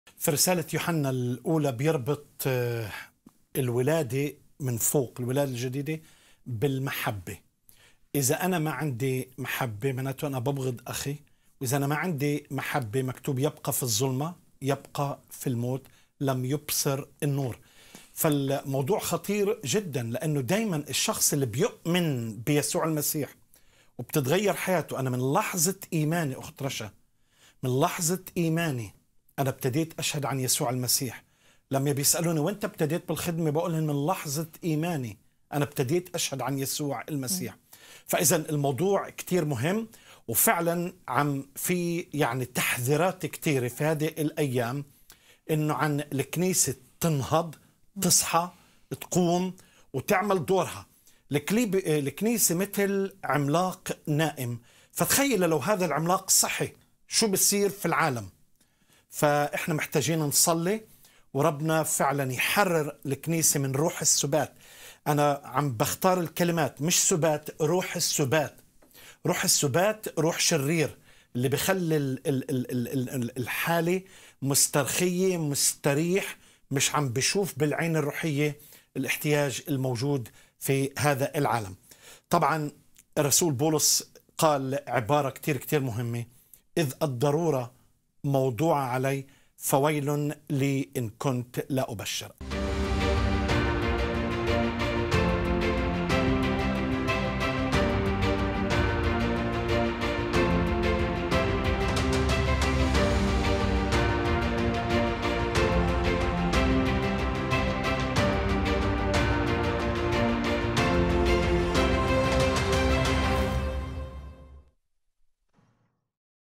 463 الكنيسة مثل عملاق نائم .. فماذا لو استيقظ؟
عزيزي المشاهد
عنوان هذه الحلقة : (الكرازة وسرعة مجيء الرب)
ضيفة الحلقة: الأخت/ رشا نور
يمكنك مشاهدة الحلقة كاملة بالضغط على هذا الرابط: